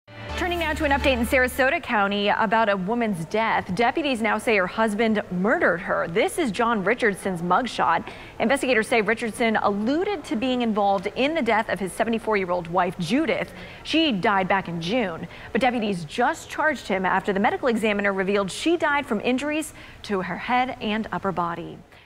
Sarasota County man charged with murder after wife's death
A 76-year-old man in Sarasota County is facing second-degree murder charges in the death of his wife, according to the sheriff's office.